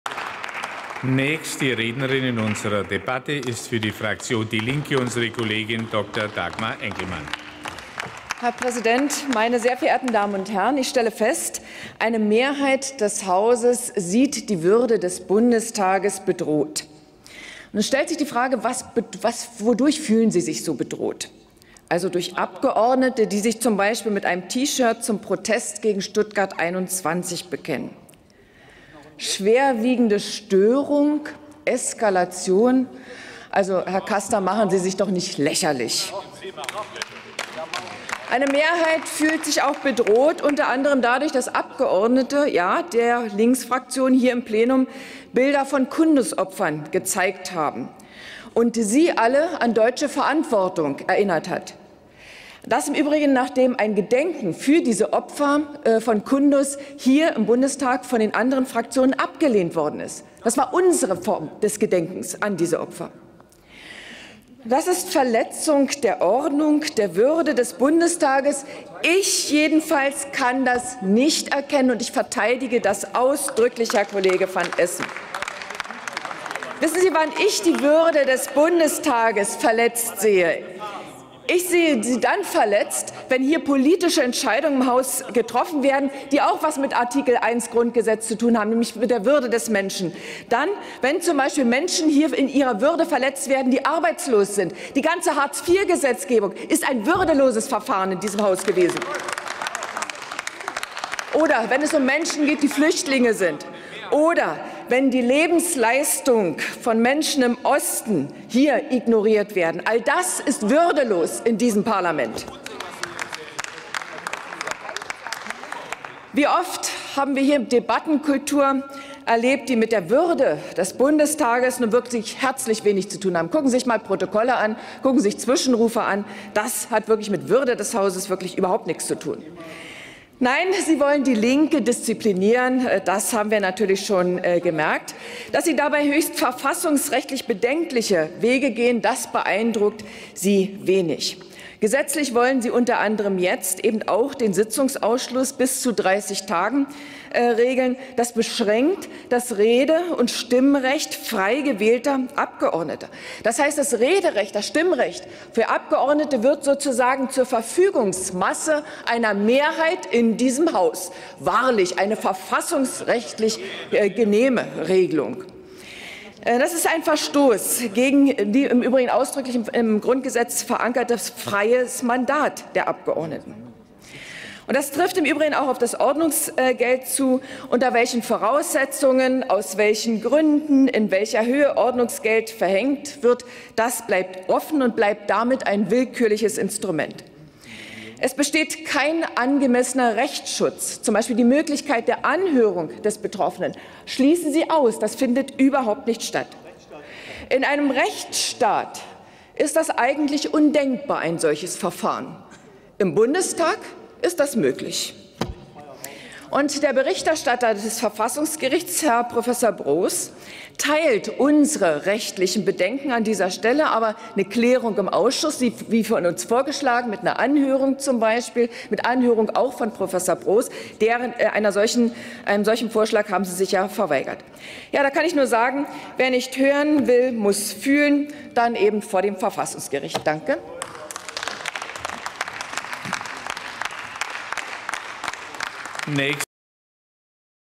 Dagmar Enkelmann, DIE LINKE: Rede zur Änderung der Geschäftsordnung des Deutschen Bundestages
Ordnungsgeld gegen Abgeordnete verletzt die Würde des Parlaments und die verfassungsmäßigen Rechte des Abgeordneten; kein angemessener Rechtsschutz gegen die Sanktionen